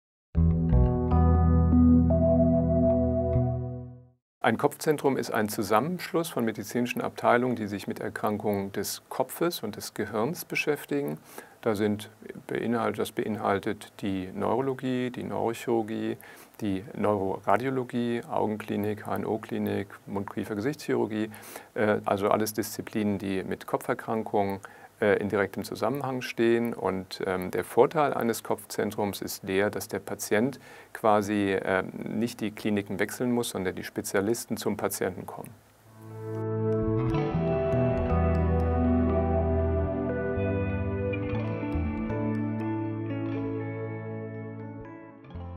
Was ist ein Kopfzentrum? - Medizin ABC | Asklepios
In einem Kopfzentrum arbeiten Ärzte interdisziplinär, denn gerade Erkrankungen im Kopfbereich gehen häufig mit Funktionsstörungen in mehreren Bereichen einher. Dabei sorgt die fächerübergreifende Versorgung für eine optimale Behandlung der Patienten. Prof. Dr. Günter Seidel erklärt, was ein Kopfzentrum ist.

Prof. Dr. Günter Seidel, Chefarzt für Neurologie und Neurologische Frührehabilitation an der Asklepios Klinik Nord – Heidberg